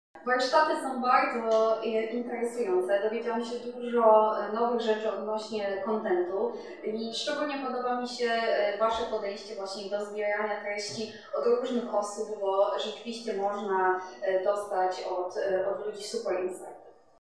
Agata Kapica z Microsoft o warsztatach MillionYou
Warsztaty "Jak stworzyć angażujący content video i skutecznie trafić do widzów w sieci?" organizowane przez MillionYou i  Grupę Onet. Partnerzy: Media Impact Polska, Pictura.tv, The Boom.

O smakowite poczęstunki i podniebienie gości dba Batida.